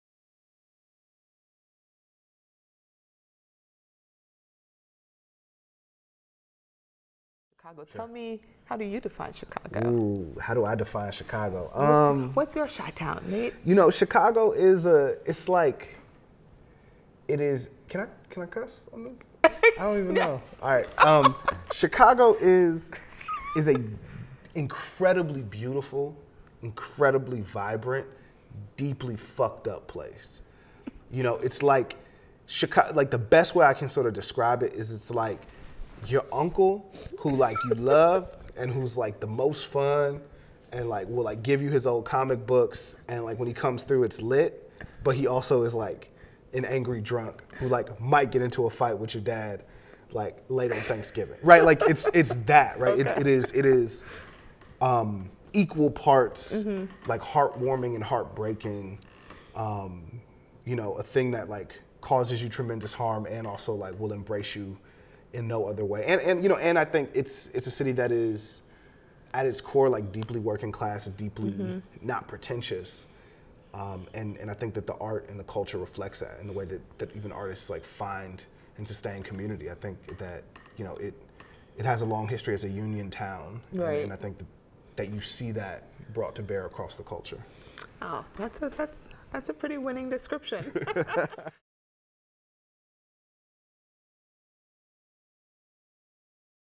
The Fight & The Fiddle: Nate Marshall on Chicago
In an interview with editor-in-chief, Lauren K. Alleyne, poet Nate Marshall defines his hometown of Chicago. Read the full interview, debut poems, and critical analysis at fightandfiddle.com.